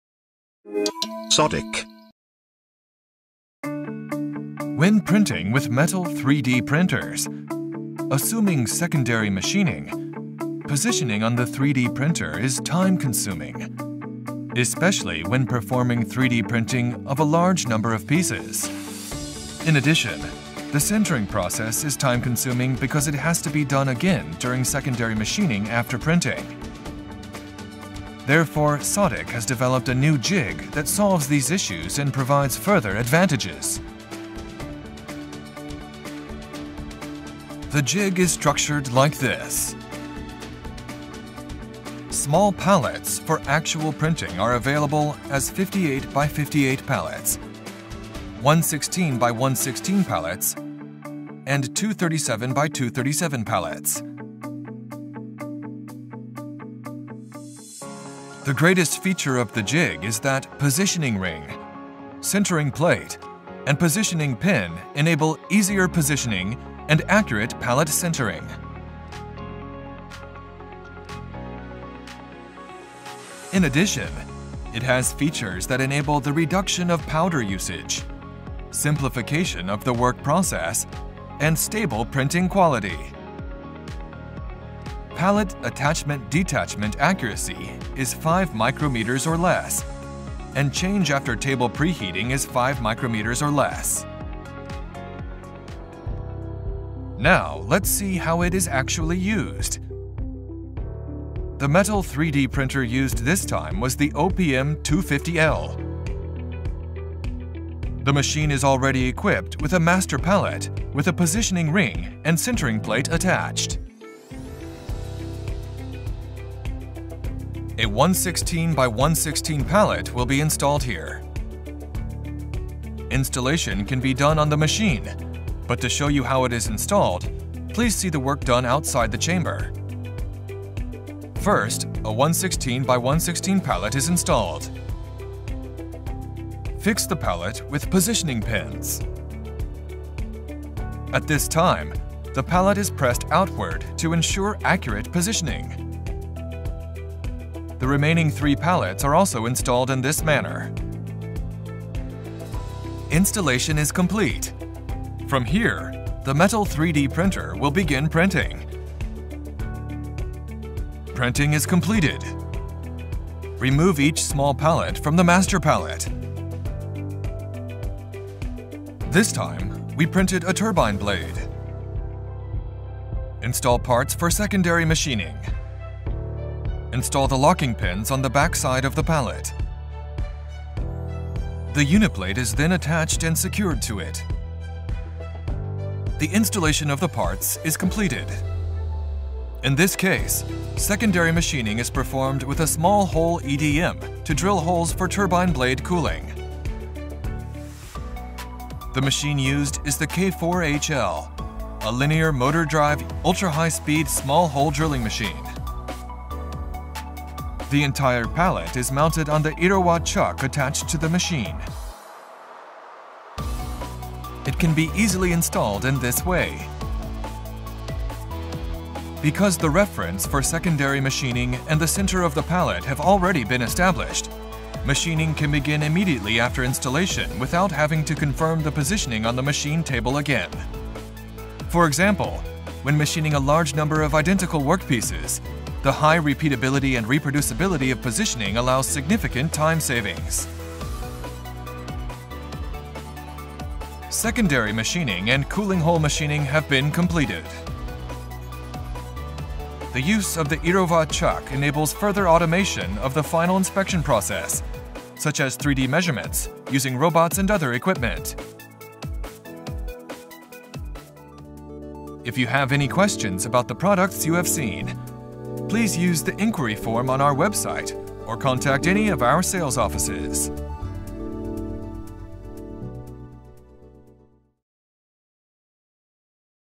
Sodick OPM additive manufacturing
The Sodick OPM vastly outperforms laser-only printers in the production of complex parts, slashing manned post-process operations, replacing them with a seamless unmanned process.